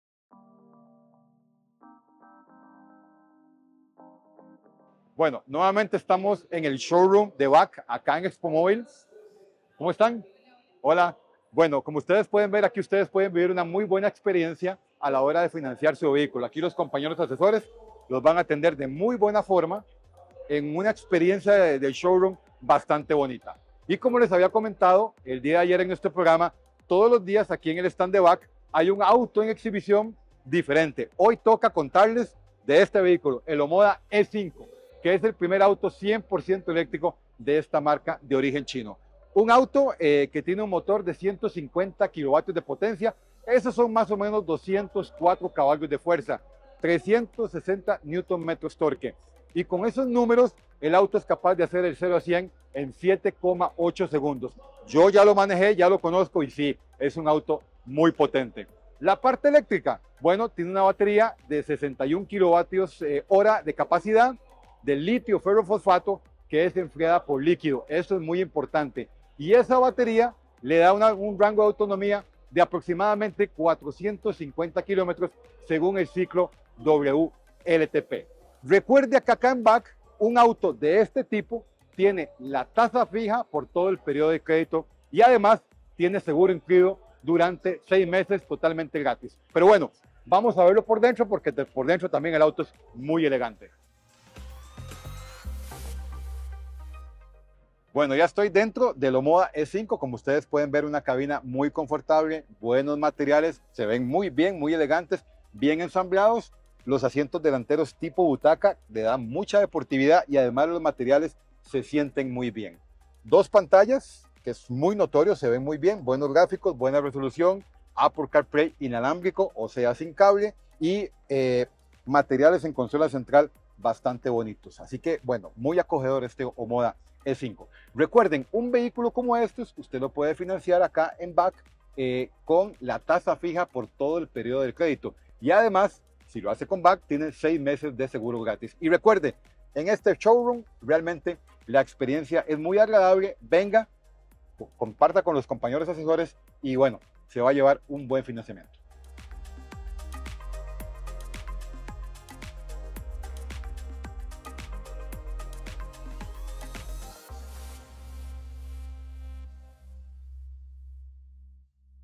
EXPOMOVIL 2024 : OMODA E5
DESDE EL SHOWROOM BAC EN EXPOMOVIL, ESTE ES EL NUEVO OMODA E5, PRIMER SUV ELECTRICO DE LA MARCA